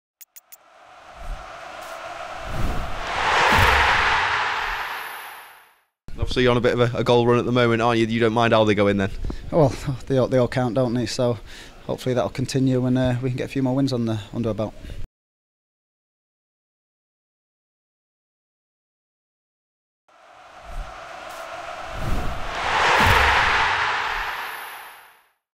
Freeview: Ajay Leitch-Smith Post Blackpool (A) 2015/16
A clip of Port Vale striker Ajay Leitch-Smith's post-match ValiantPlayerHD interview following the win over Blackpool.